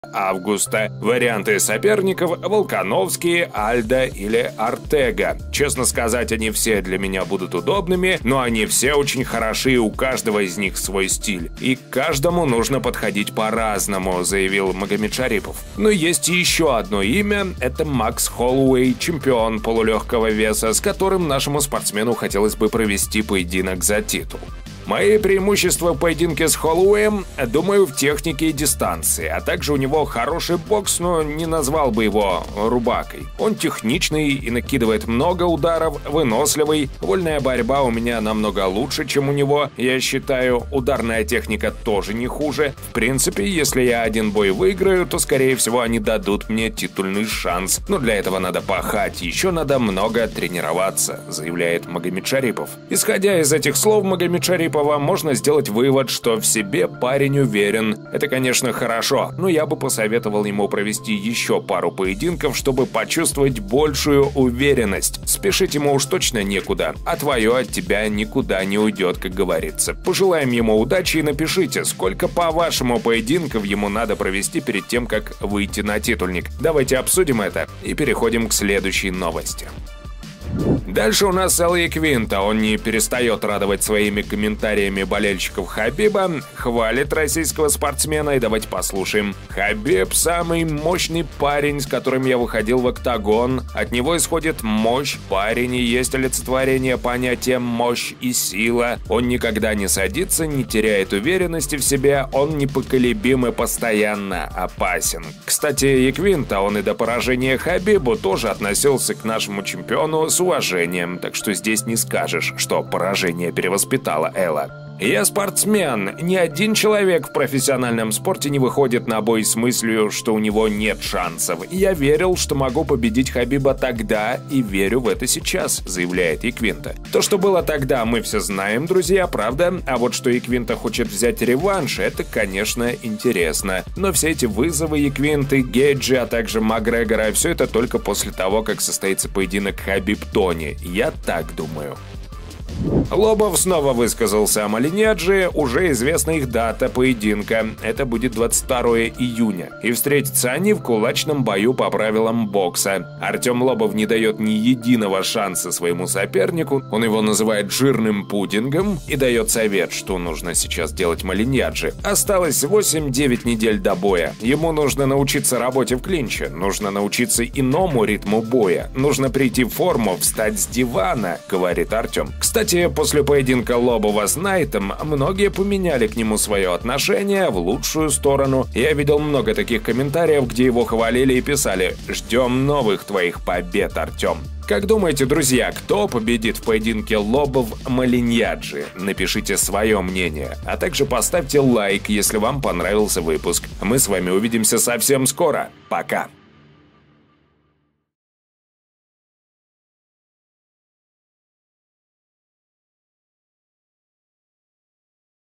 МАГОМЕДШАРИПОВ СРАВНИЛ СЕБЯ С ЧЕМПИОНОМ МАКСОМ! ФОРМА И НАВЫКИ НА УРОВНЕ! ЯКВИНТА О ХАБИБЕ
МАГОМЕДШАРИПОВ О СВОЕЙ ЦЕЛИ И ЧЕМПИОНСКОМ ТИТУЛЕ! ЯКВИНТА РАССКАЗАЛ О НУРМАГОМЕДОВЕ

В этом выпуске послушаем слова Эла Яквинты о Хабибе, он похвалил чемпиона. А еще послушаем слова Артема Лобова о боксере Малиньяджи.